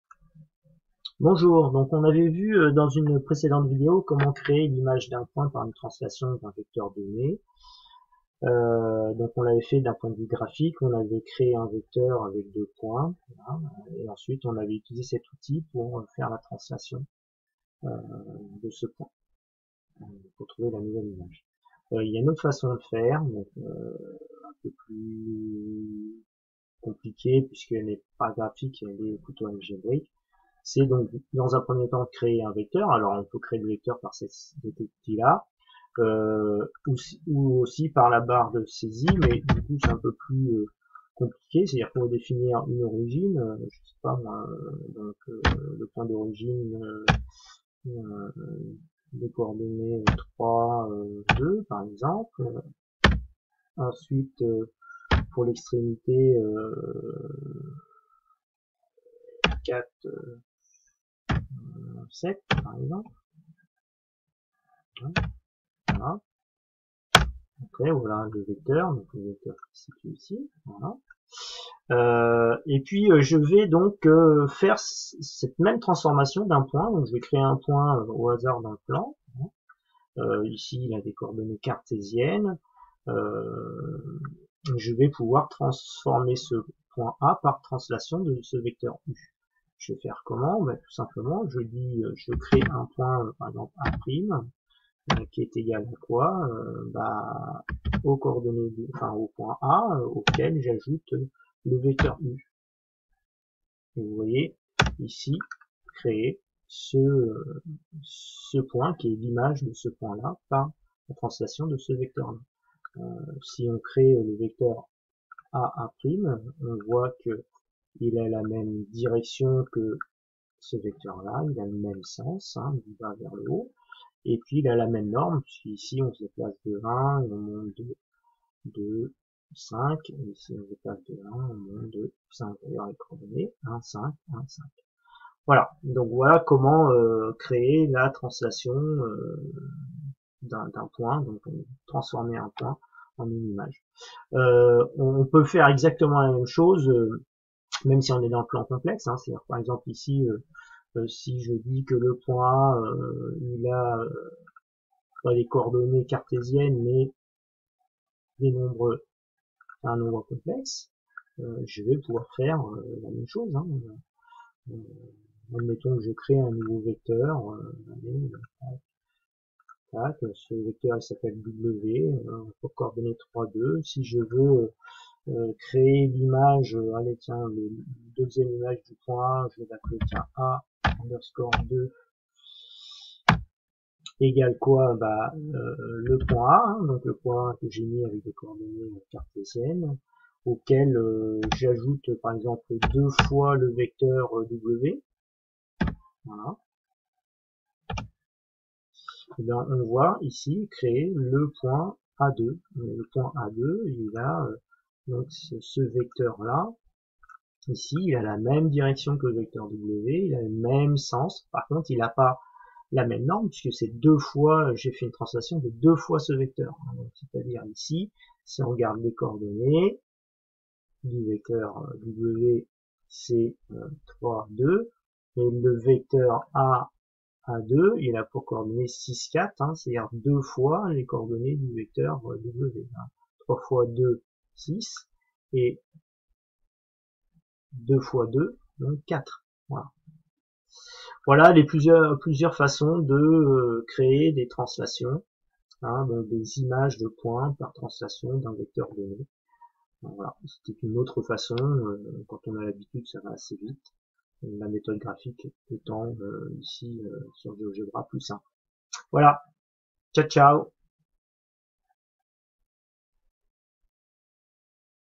translation algébrique
Cette vidéo à pour but de montrer comment créer avec GEOGEBRA l'image d'un point par translation de vecteur donné en utilisant une construction algébrique.